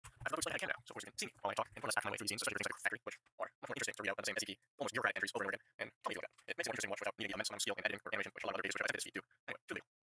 Webcam Exposition speedrun .mp4
Clipped out of a post-editing recording of the increasingly intricate 'Reading off all of SCP' series for 002-100

Edit: it's for some reason, more like 0.5x speed despite the editing speed being 4x, now I know I suppose
Quality is choppy due to a low-quality export, I just made it and threw it online, alternative is of course, 75 random Clips that noone will watch anyway!